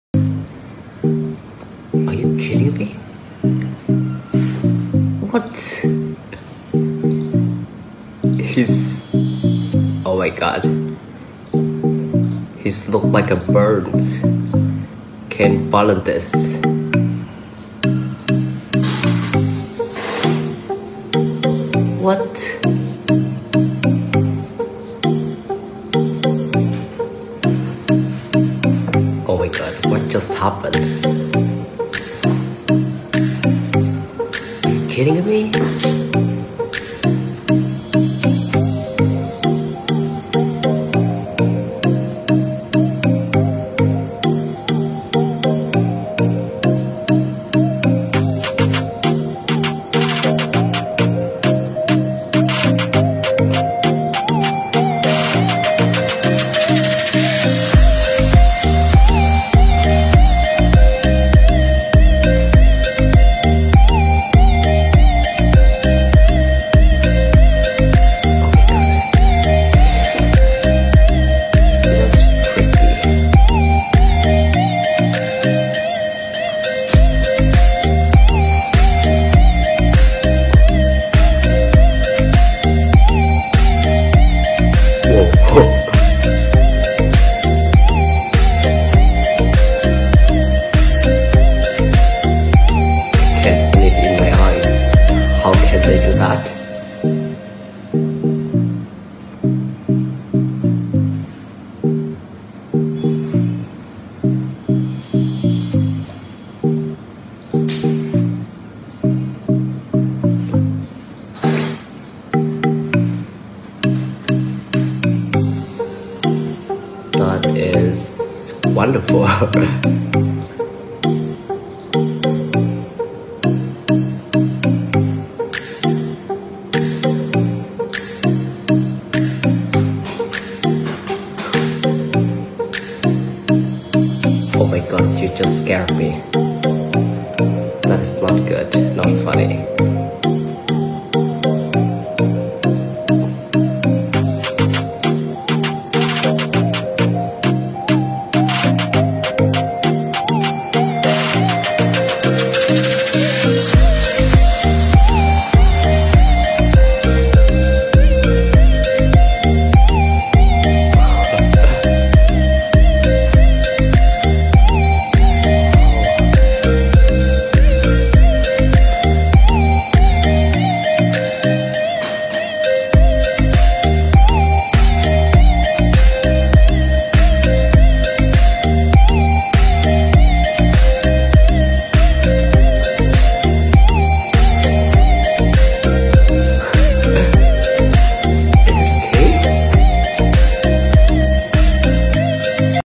Talented artist ➖ balance in a rope😱 How ⁉️ Adam bartley
Real talent and very flexible balancing the whole body
📌It's amazing. How do you practice? Please tell, whats the story of them big puppys .🤭🤭